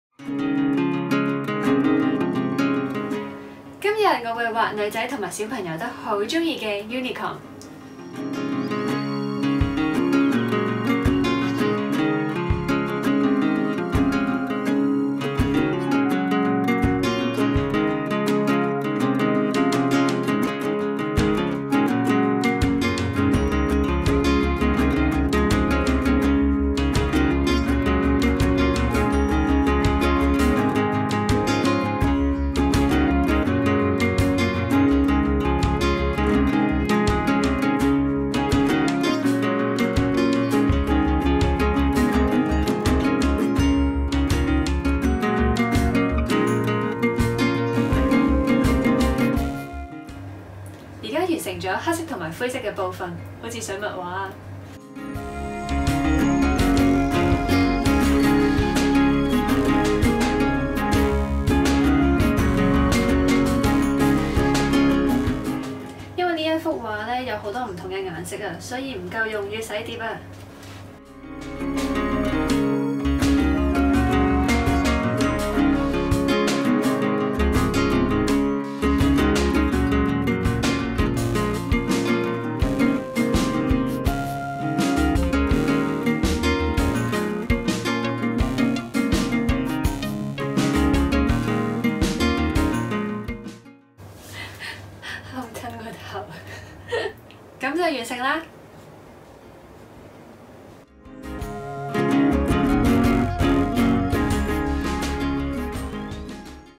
Jan姐-謝芷倫 Watercolor speed painting | Unicorn 用水彩畫獨角獸
最近幾鍾意畫水彩畫，相比起畫 acrylic 畫，水彩畫會畫得快d，畫畫減壓都唔錯。